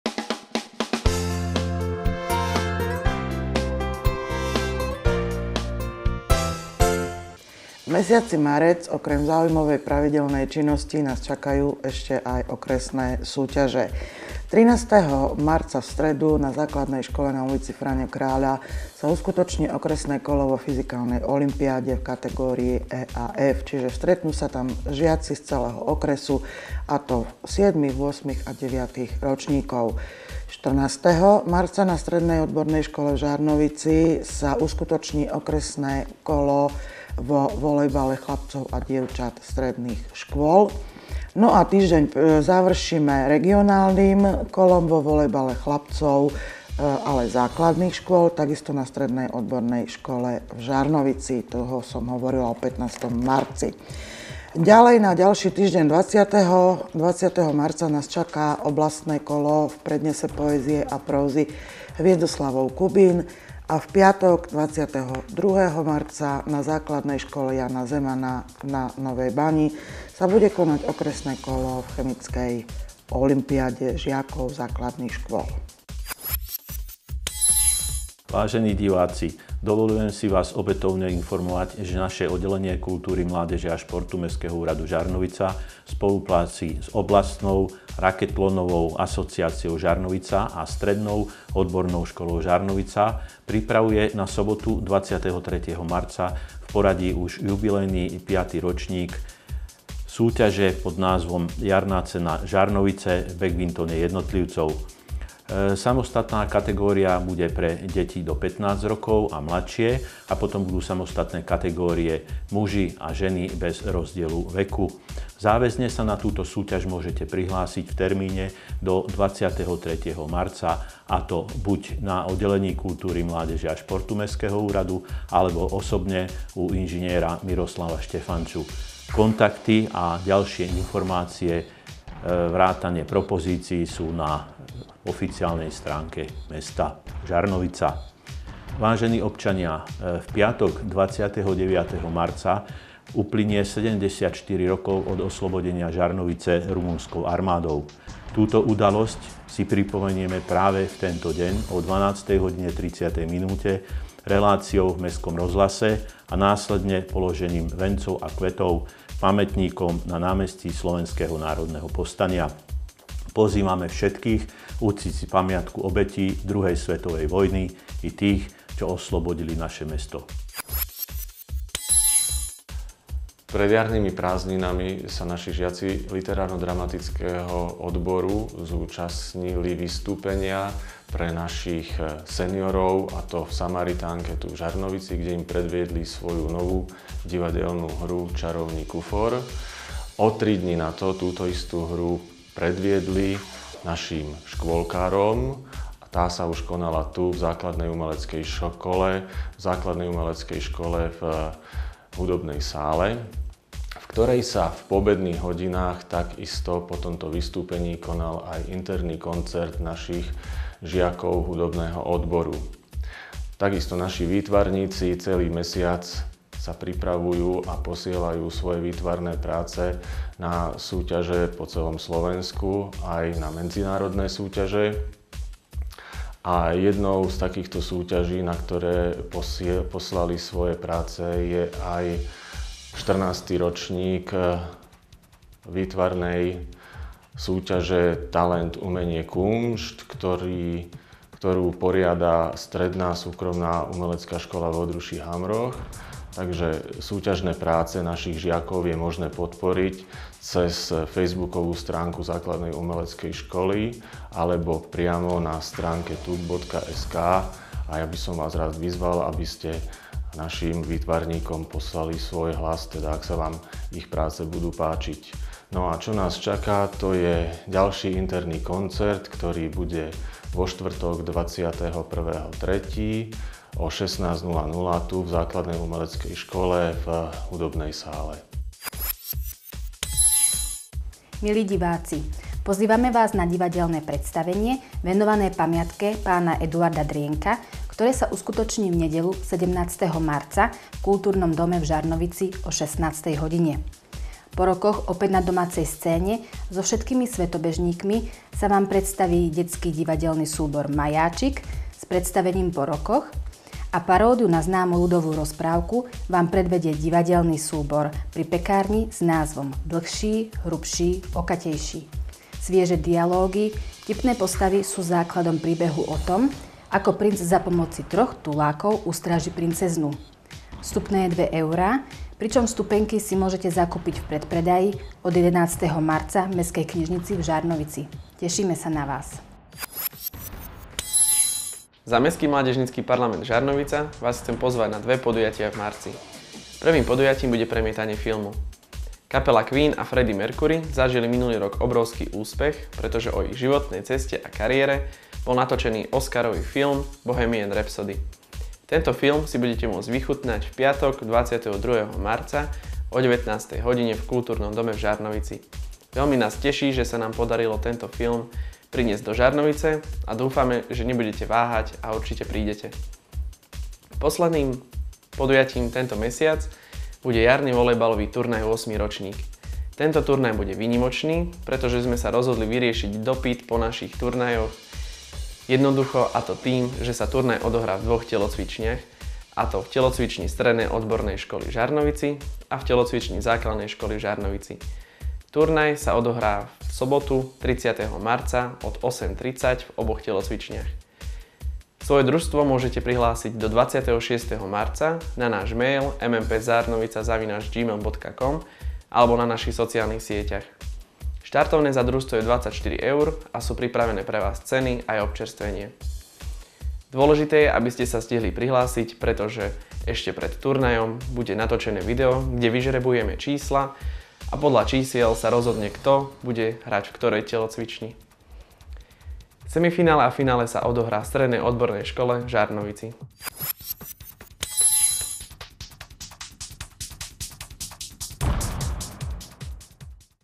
Monitor 5/2019
Relácia o pripravovaných aktivitách a akciách mesta Žarnovica na mesiac marec.